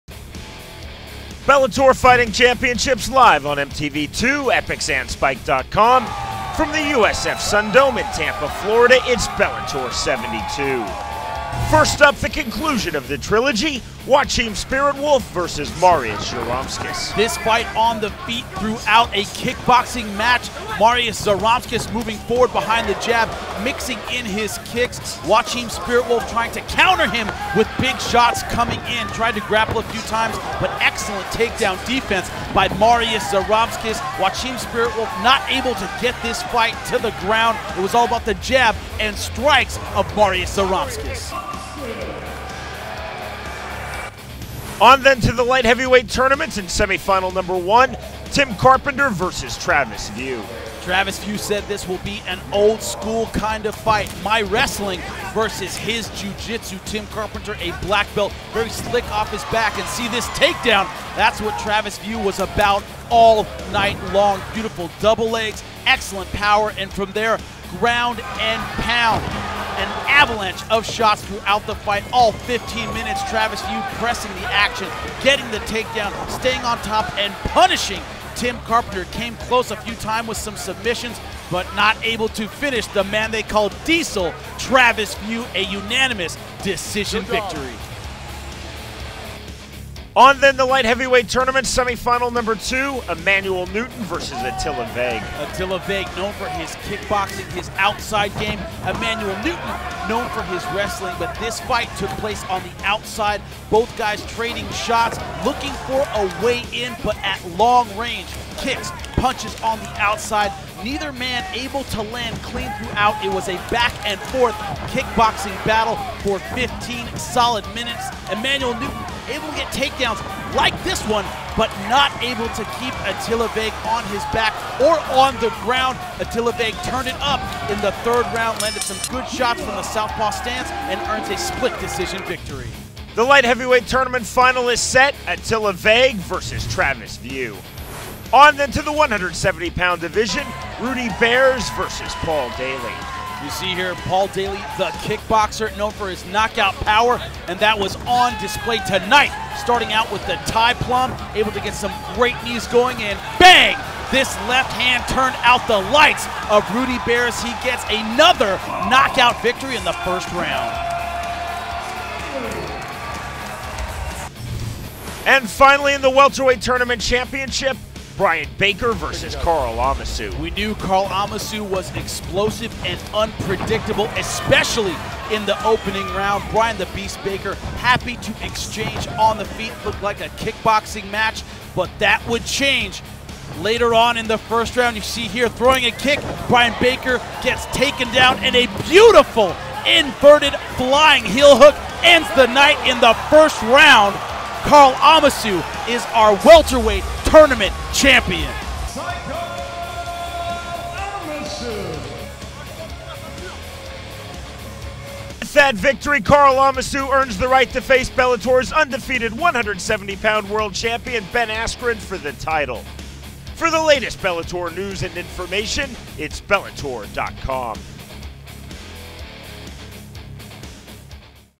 Bellator MMA Highlights: Fantastic Finishes
Bellator MMA - July, 20, 2012: In a night of fantastic finishes, Karl Amoussou submits Bryan Baker with a heel hook out of no where to earn a shot against Bellator MMA welterweight champion Ben Askren! Plus: Attila Vegh edges out Emanuel Newton to win the Light Heavyweight Tournament and Paul Daley makes his Bellator debut in typical "Semtex" fashion!